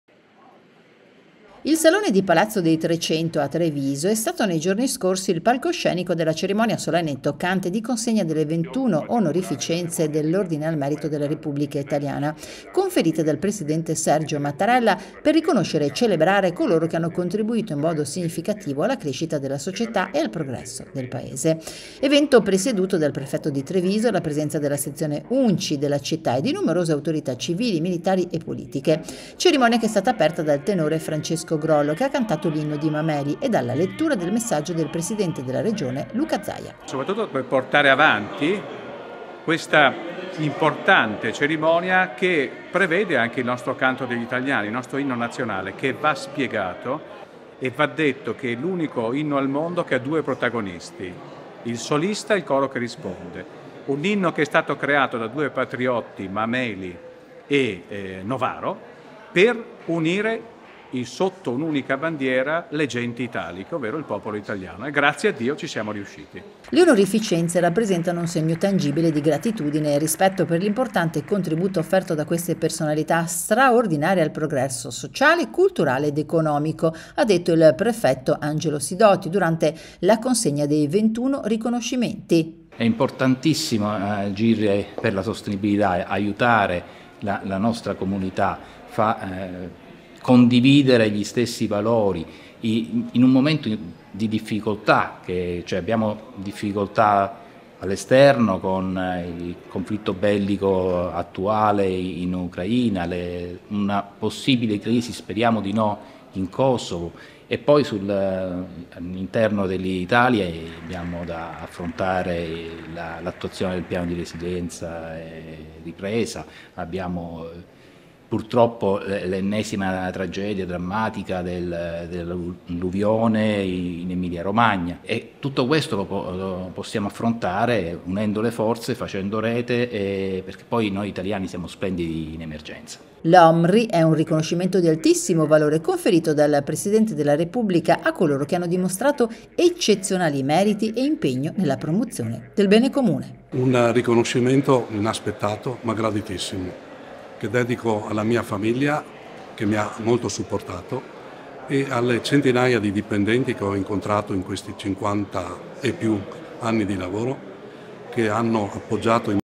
DISPONIBILITA DA PREMIARE
A Treviso, sono stati nominati 15 nuovi cavalieri della Repubblica. Consegnati anche i Premi UNCI “Città di Treviso” a Carabinieri, Guardia di Finanza, Polizia e Vigili del Fuoco.